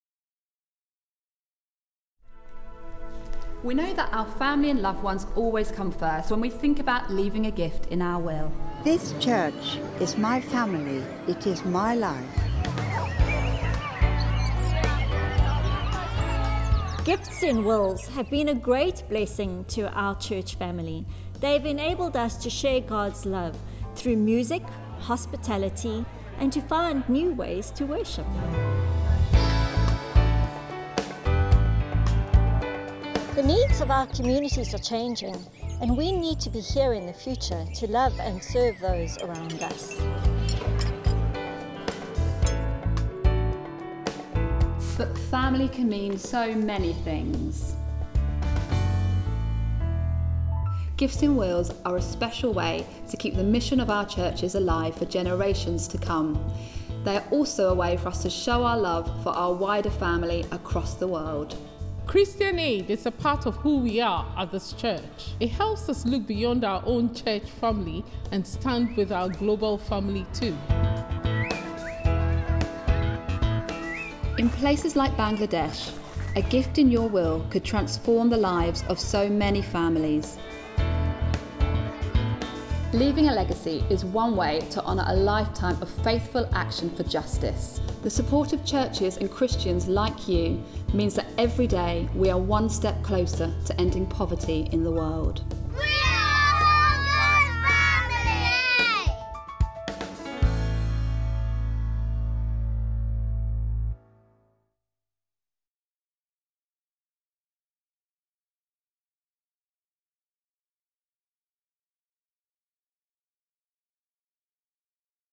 Leaving a Legacy CA
Kerry from Christian Aid asks about the legacy we leave behind...